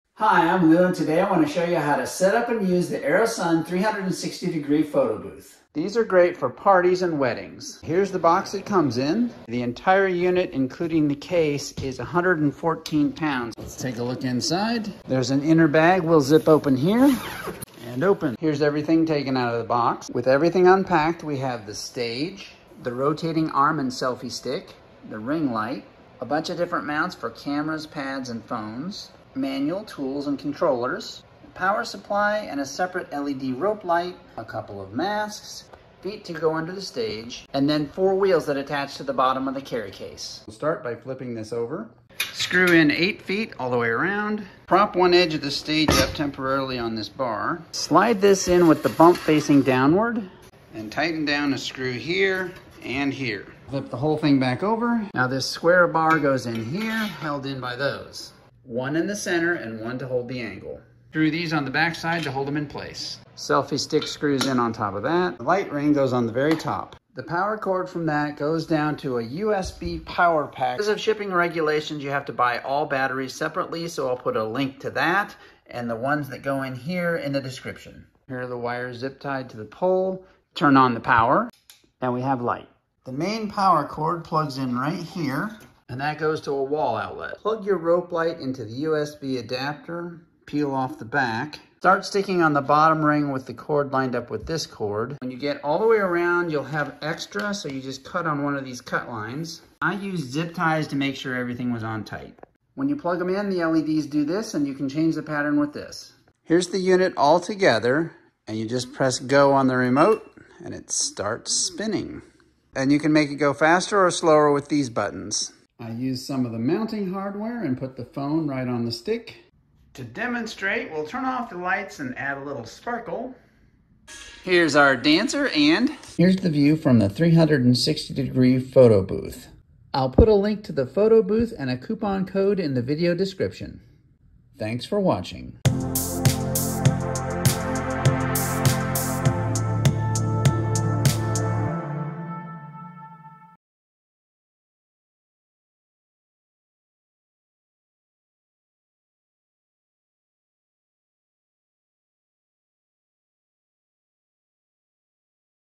Airusan 360 Photo Booth: Setup and Use
These photo booths are common at weddings or large gatherings.  They are easy to set up and use and provide an exciting new look to your videos.

Battery Pack for Ring Light

Batteries for all remotes: